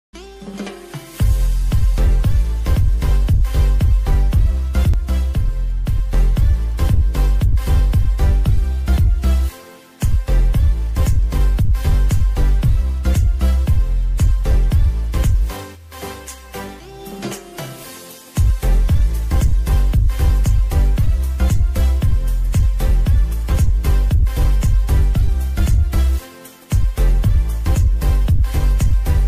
lampu viral dari barang bekas.‼️
ide kreatif cara membuat lampu tidur, lampu hias, lampu belajar dari barang bekas